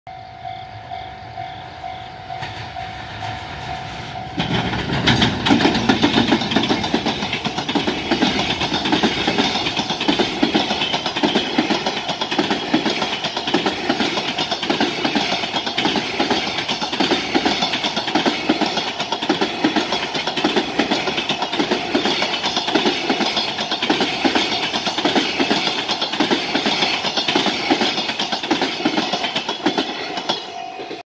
東北本線の貨物列車 4085レ 高速貨物B 広島ター仙台タ
金太　日陸MDI、JOTシジクロペンタジエン

9/25